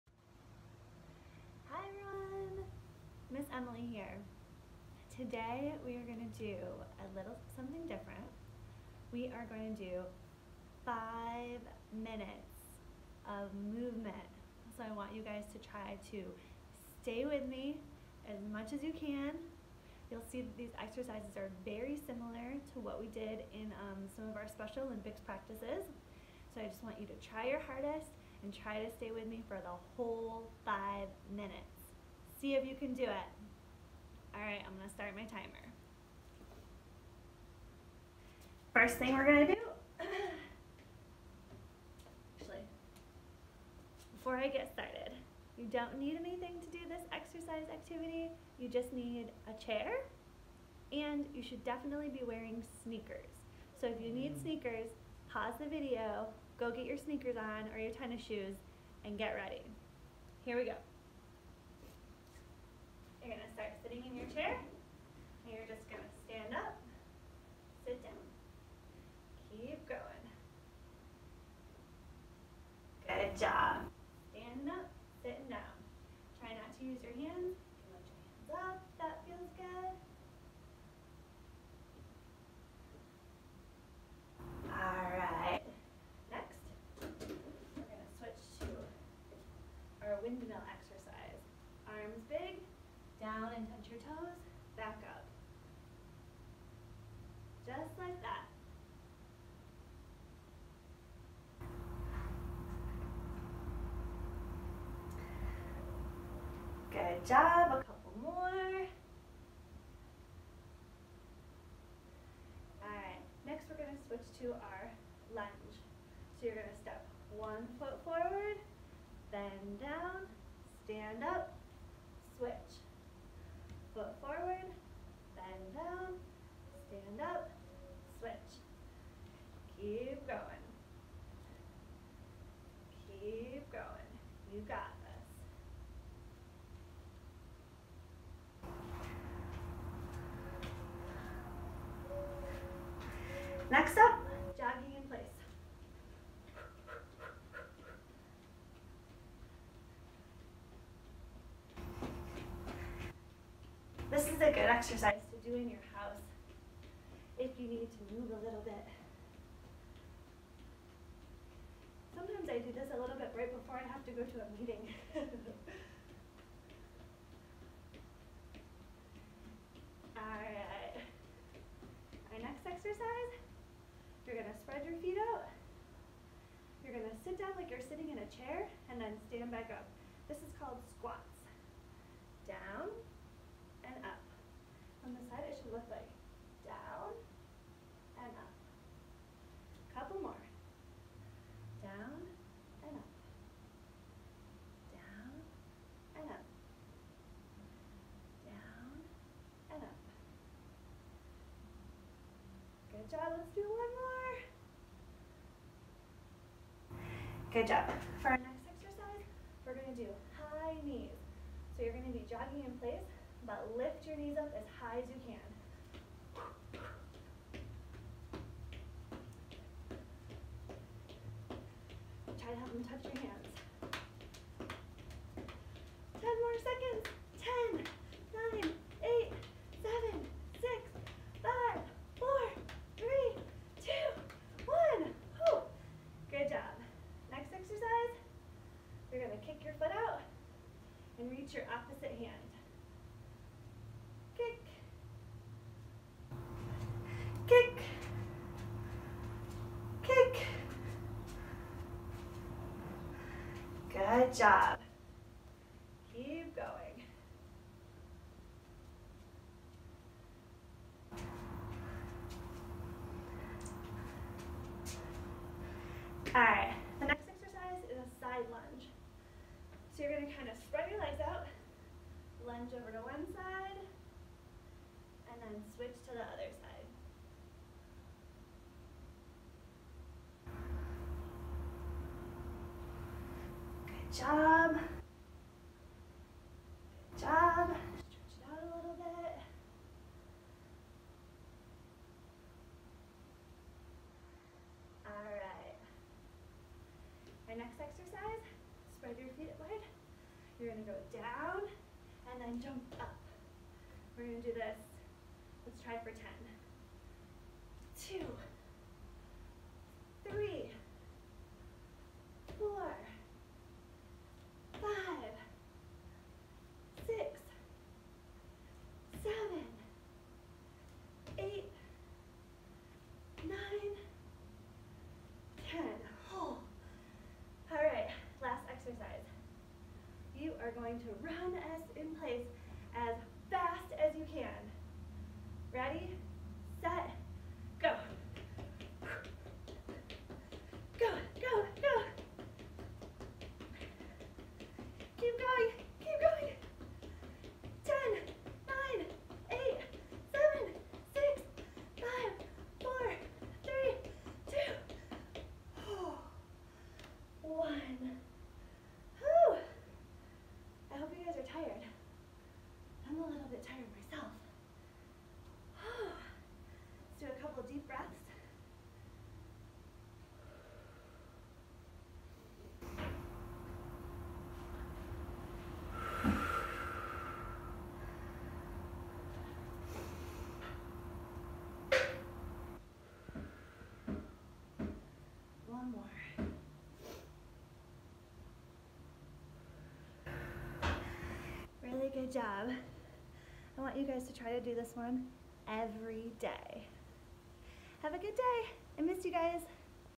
5 Minutes of Movement
Great way to start your day! This is a great video for children who are standing/jumping independently. A lot of these exercises the students will be familiar with from Special Olympics practice or yoga. Just a reminder these videos are recommended for students I know from class/special olympics in Phoenix AZ. Feel free to give it a try, just use caution.